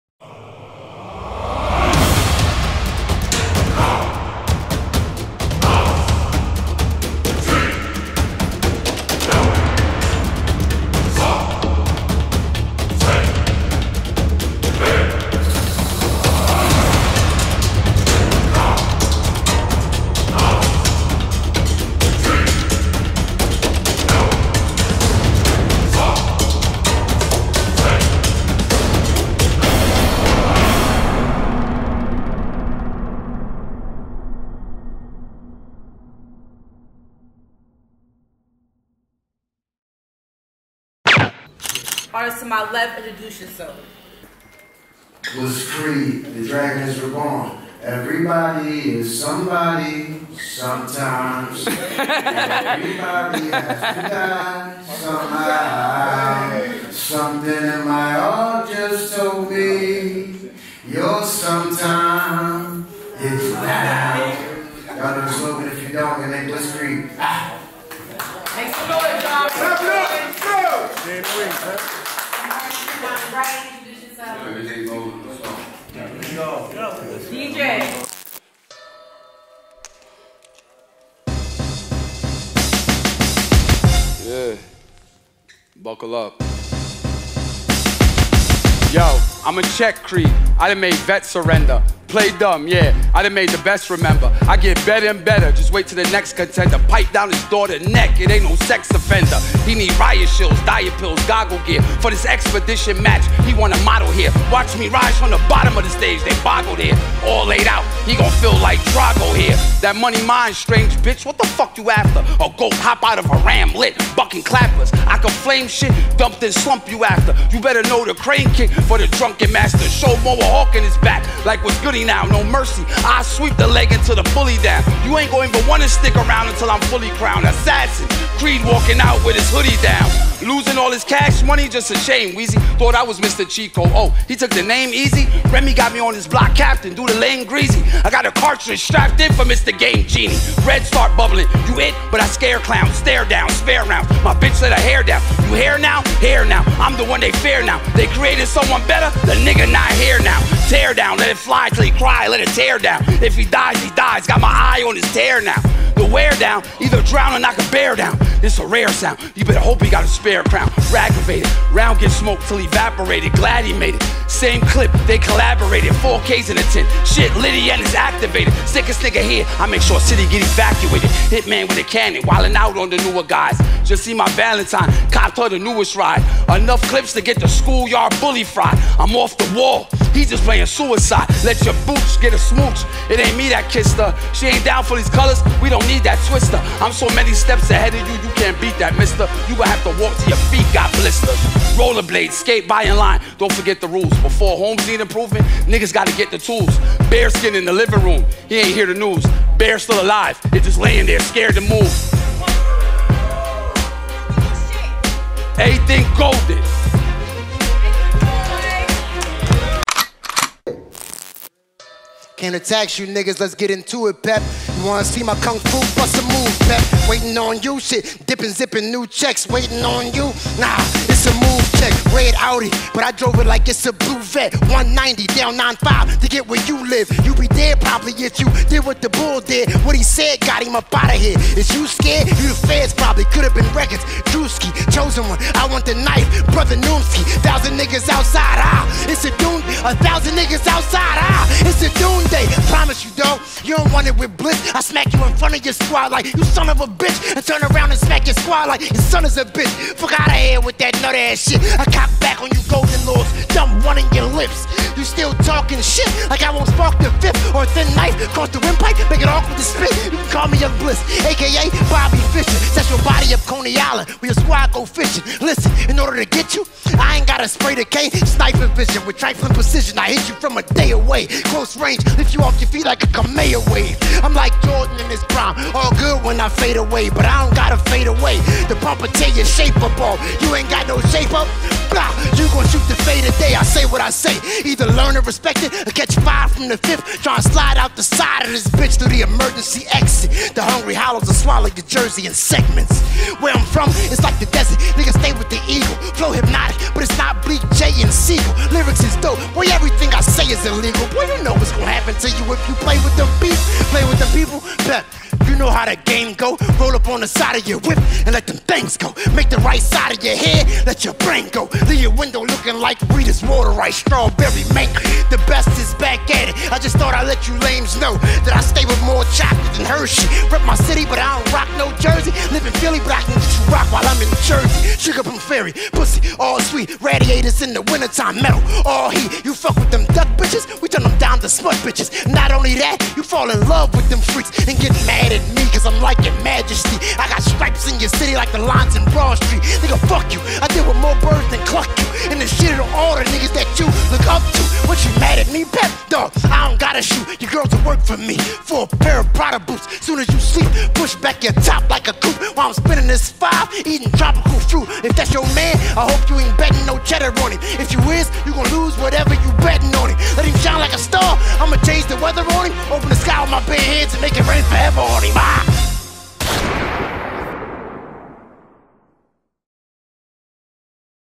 [VERBAL WAR ZONE] PEP VS BLISS CREED "STIMULUS"
OG CHOSEN AND TRUE BATTLE RAP IN CONJUCTION WITH VERBALWARZONE PRESENTS STIMULUS. 32 RAPPERS GO HEAD-TO-HEAD FOR TOP PRIZES OF 5K AND 1K.

VIDEO DIRECTORS:
XSTRUNDARE
B CITY WEGOHARDTV
YABOYCLIP FLIMS

HOSTED BY:
CHOSEN OG, NADIALI, AND SHELOVESBATTLERAP.

JUDGES:
SWAVE SEVAH "RESTORE ORDER"
HURRICANE DU
KNOWLEDGE THE GOD "HIP HOP IS REAL"
T REX
SARA KANA